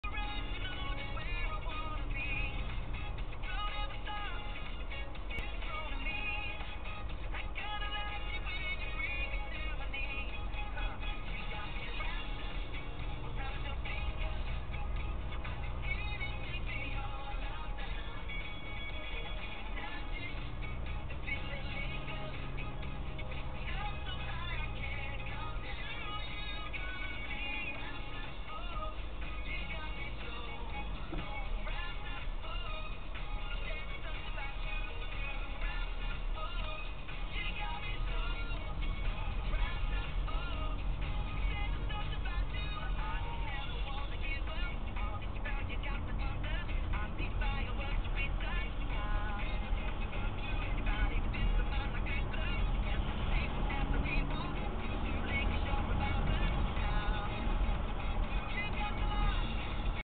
Derbyshire vs Staffordshire in the snow
Derbyshire do not grit or plough in the mornings, Staffordshire plough & grit and road is clear. Taken on road from Mony Ash, Derbyshire to Longnor, Staffordshire at the bottom of the hill in Crowdecote. Apologies upside down!